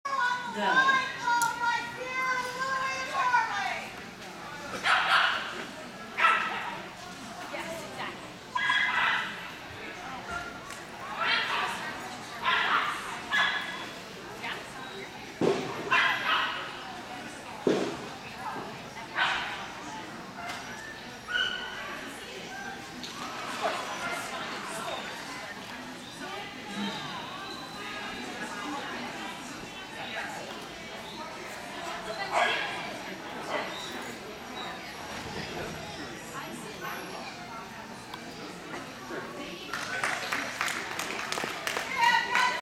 Score - Excellent B JWW, DMOTC, March 19th, 2011
Popped out of the weaves - too much pressure on him before that wrap after the weaves.